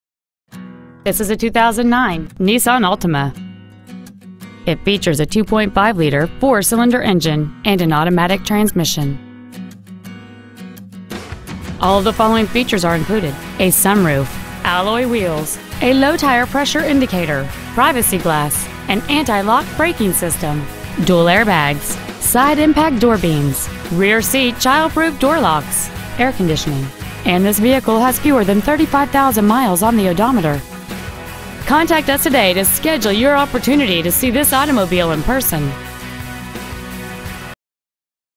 2009 Altima Bristol CT 06010
Year : 2009
 Make :
 Model : Altima
 Engine : 2.5L 4 Cyl. Fuel Injected
 Trans . : Automatic
 Exterior : Gray
 Miles : 34,073

 Used 2009       Altima Bristol CT